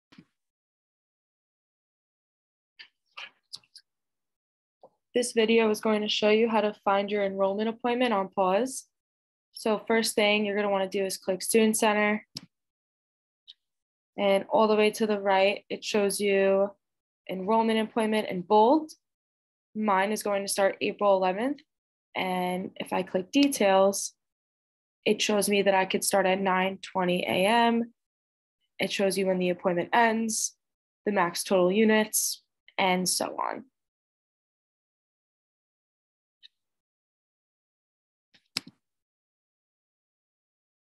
How to find your enrollment appointment in PAWS
As a student, you're assigned a day and time when you can enroll in classes. This tutorial shows you how to locate that information.